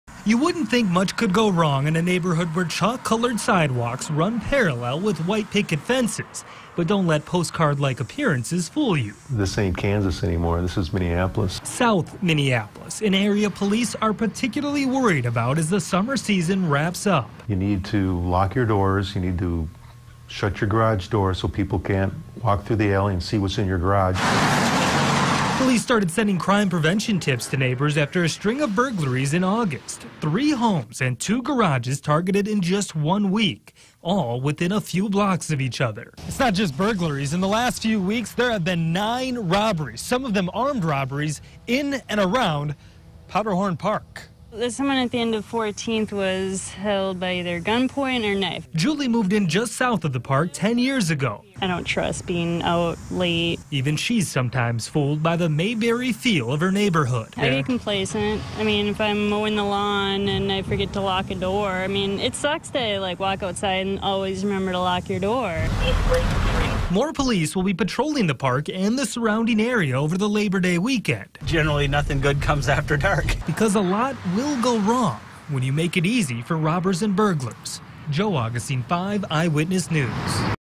Police Increasing Patrols Near Powderhorn Lake
For people in south Minneapolis neighborhoods, police say they are tracking an increase of homes being burglarized and people being robbed. The police are especially focused on Powderhorn Lake.